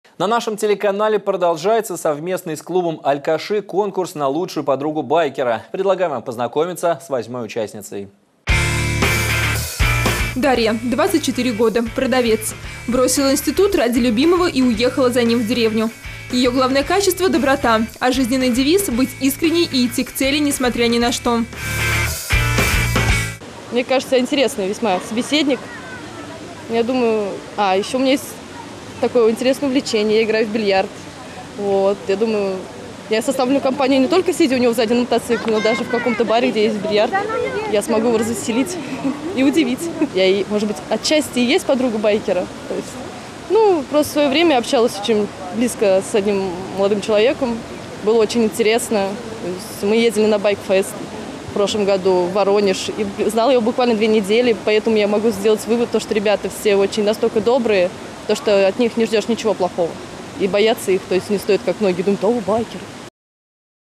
Восьмая участница конкурса на лучшую подругу байкера
На нашем телеканале продолжается совместный с клубом "Аль-Каши" конкурс на лучшую подругу байкера. Предлагаем вам познакомиться с восьмой участницей. Материал вышел в эфир 4 августа 2015 года.